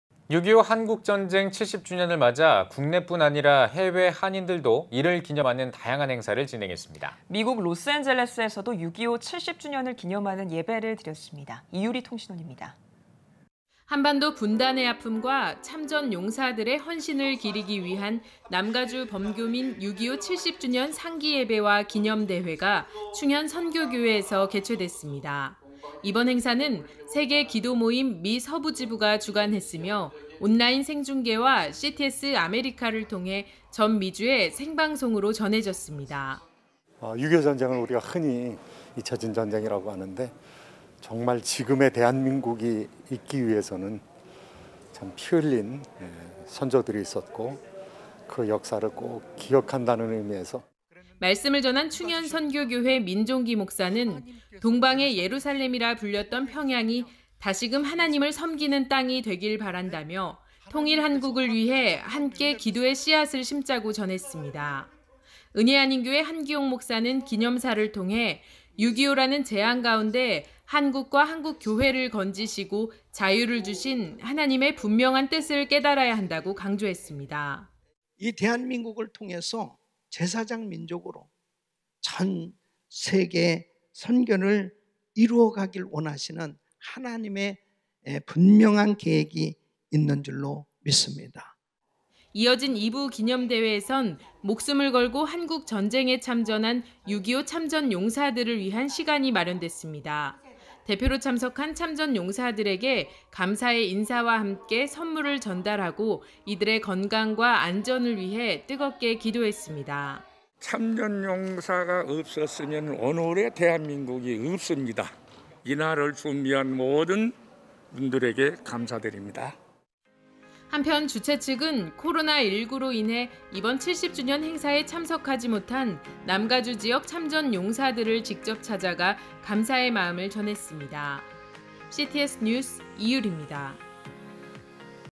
남가주 범교민 6.25 70주년 상기예배 (LA, 이율희) l CTS뉴스
앵커 : 6.25 한국전쟁 70주년을 맞아 국내뿐 아니라 해외한인들도 이를 기념하는 다양한 행사를 진행했는데요.

앵커 : 미국로스앤젤레스에서도 6.25 70주년을 기념하는 예배가 드려졌습니다. 이율희 통신원입니다.

한반도 분단의 아픔과 참전용사들의 헌신을 기리기 위한 남가주 범교민 6.25 70주년 상기예배와 기념대회가 충현선교교회에서 개최됐습니다.

이번 행사는 세계기도모임 미서부지부가 주관했으며, 온라인 생중계와 CTS America를 통해 전미주에 생방송으로 전해졌습니다.

Int 민종기 목사 / 충현선교교회

말씀을 전한 충현선교교회 민종기 목사는 “동방의 예루살렘이라 불렸던 평양이 다시금 하나님을 섬기는 땅이 되길 바란다.”며, “통일 한국을 위해 함께 기도의 씨앗을 심자.”고 전했습니다.

은혜한인교회 한기홍 목사는 기념사를 통해 “6.25라는 재앙 가운데 한국과 한국교회를 건지시고 자유를 주신 하나님의 분명한 뜻을 깨달아야한다.”고 강조했습니다.

SOT 한기홍 목사 / 은혜한인교회

이어진 2부 기념대회에선 목숨을 걸고 한국전쟁에 참전한 6.25 참전용사들을 위한 시간이 마련됐습니다.

대표로 참석한 참전용사들에게 감사의 인사와 함께 선물을 전달하고, 이들의 건강과 안전을 위해 뜨겁게 기도했습니다.

Int 정규상 참전용사 / 90세

한편, 주최 측은 코로나19로 인해 이번 70주년 행사에 참석하지 못한 남가주지역 참전용사들을 직접 찾아가 감사의 마음을 전했습니다. CTS뉴스 이율흽니다.